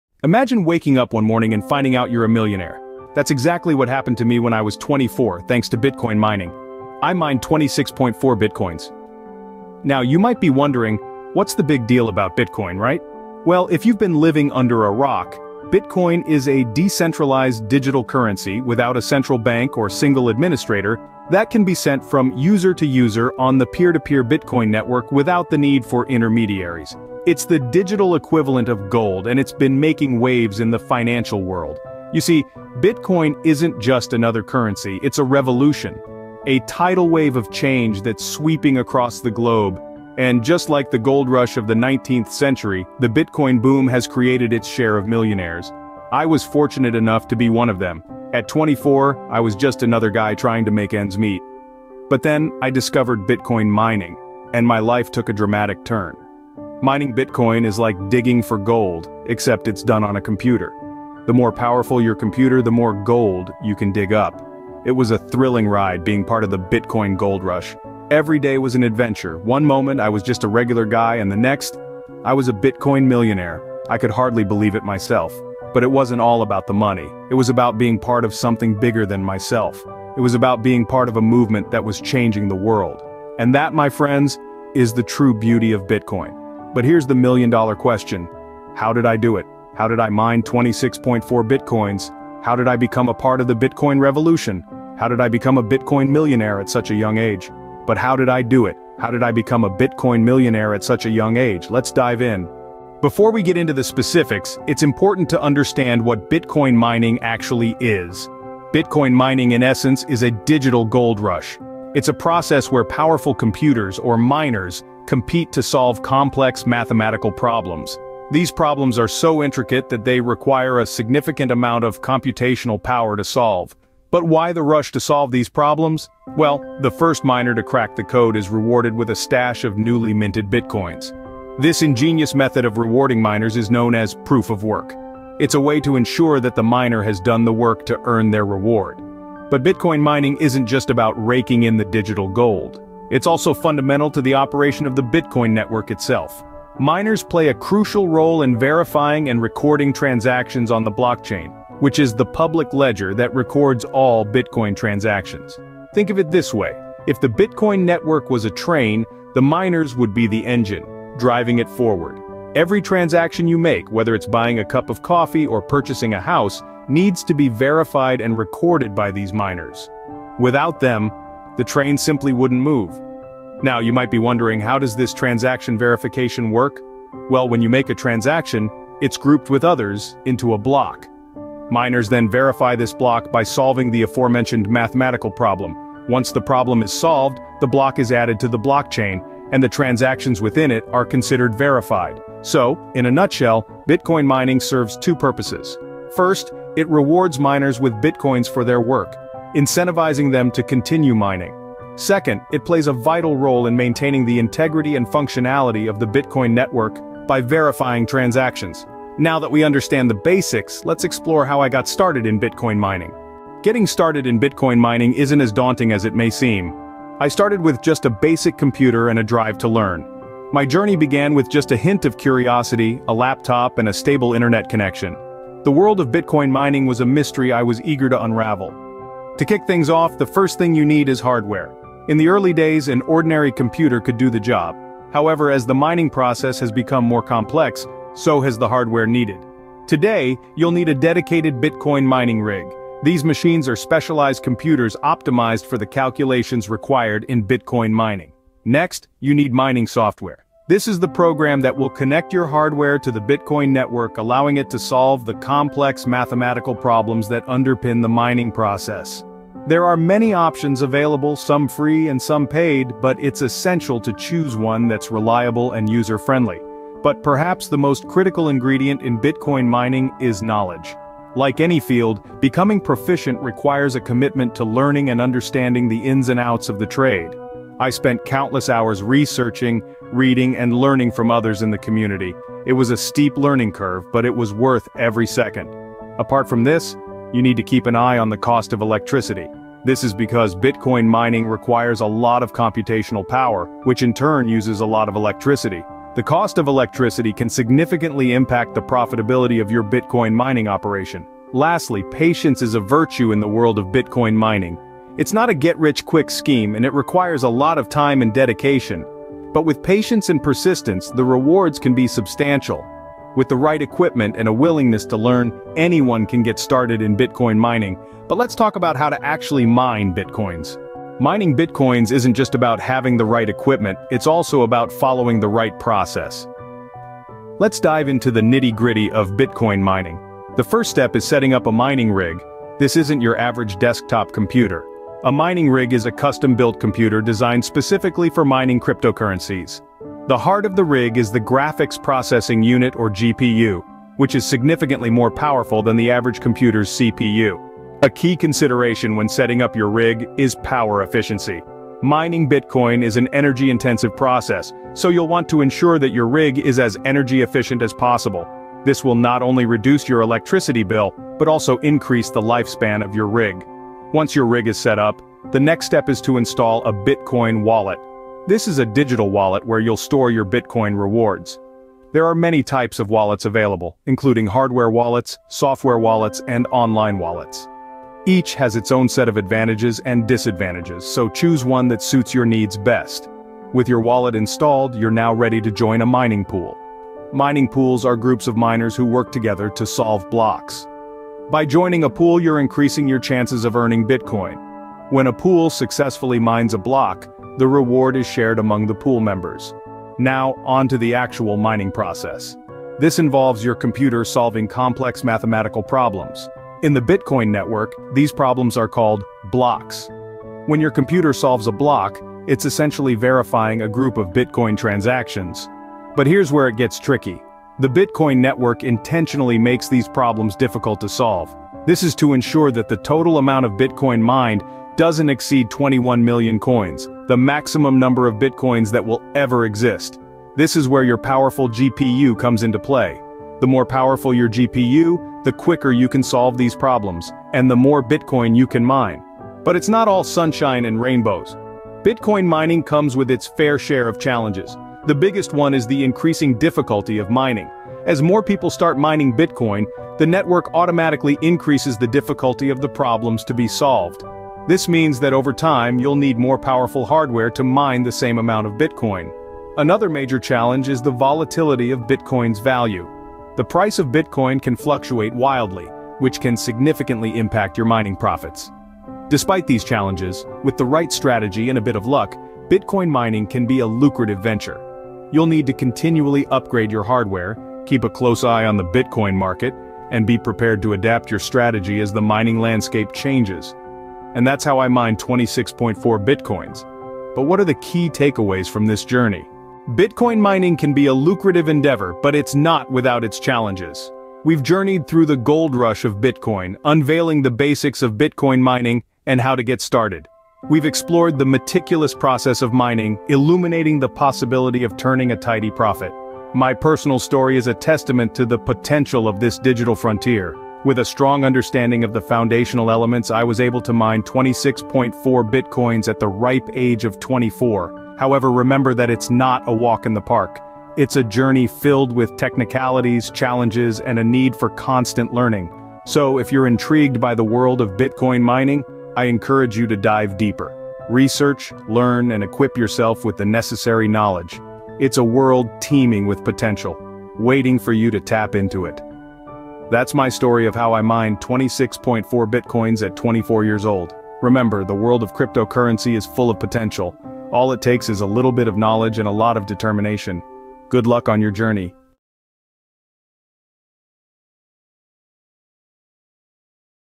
How I Mined 26.4 Bitcoins (BTC) At 24 Years Old?
In this captivating video, join me on a thrilling journey as I delve into the world of cryptocurrency mining and showcase my personal experience of mining a whopping 26.4 Bitcoins at just 24 years old.

Discover the ins and outs of crypto mining, learn valuable tips and tricks, and explore the exciting possibilities of making money online in the realm of Bitcoin and beyond. Whether you're a seasoned crypto enthusiast or a curious newcomer, this video is packed with valuable information and inspiration.

Don't miss out on this exclusive look into the world of crypto mining!